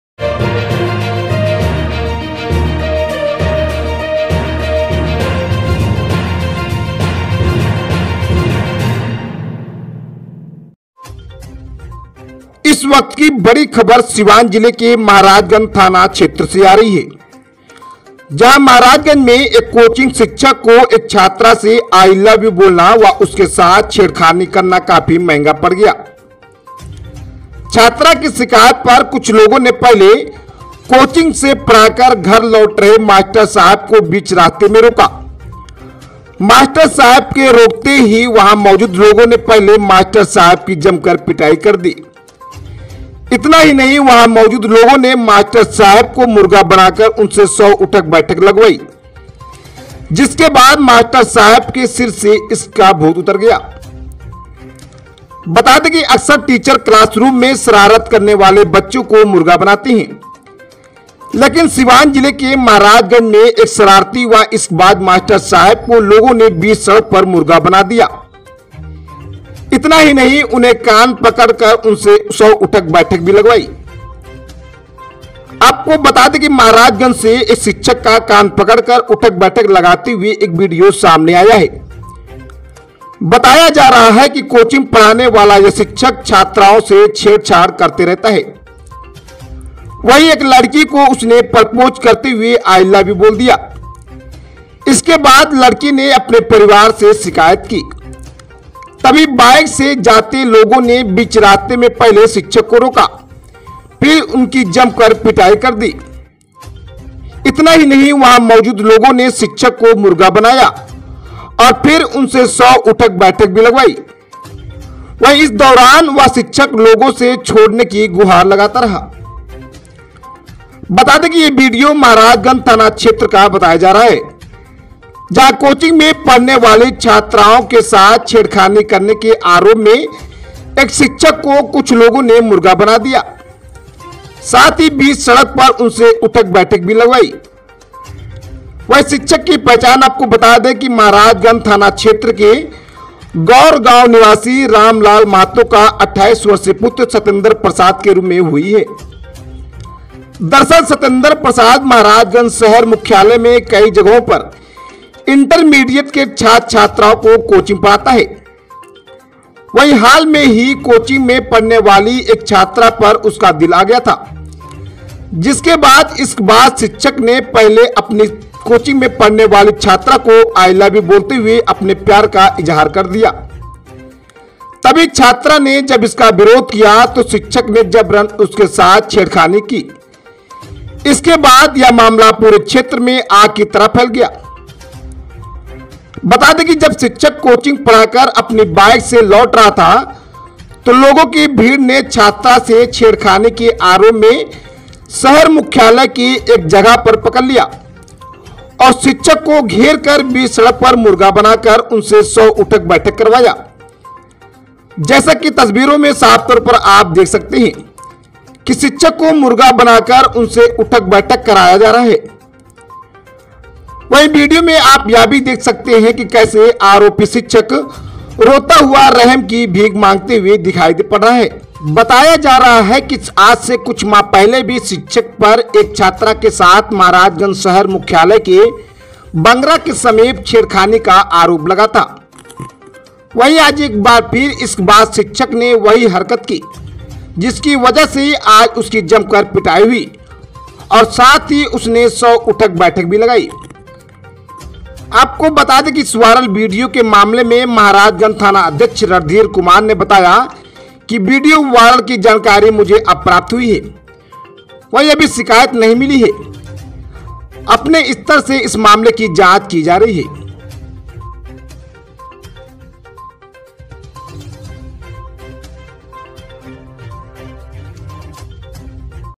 महाराजगंज में इश्कबाज शिक्षक को छात्रा से I Love you बोलना व छेड़खानी करना पड़ गया महंगाNEWSTVSIWAN
अभी-अभी महाराजगंज में इश्कबाज शिक्षक को छात्रा से I Love you बोलना व उसके साथ छेड़खानी करना पड़ गया महंगा

देखिए लोगों ने बीच सड़क पर मास्टर साहब की जमकर कर दी पिटाई व मुर्गा बनाकर कराई 100 उठक-बैठक- NEWSTVSIWAN.IN